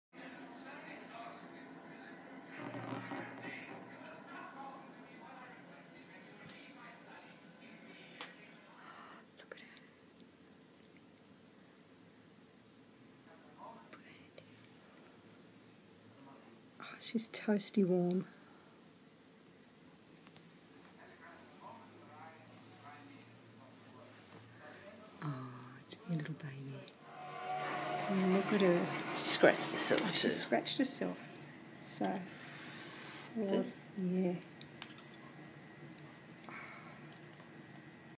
What A Difference A Week Makes.
Little Josephine continues to grow and develop. The pink graze next to her pouch is from her scratching herself. She has a fine dusting of fur coming through. She is able to be out of her humidicrib for a moment for a very quick cuddle so she can get to know her foster mum. It has been such a privilege to be able to watch her develop so far. Looking forward to watching the next stage as she begins to show her little personality.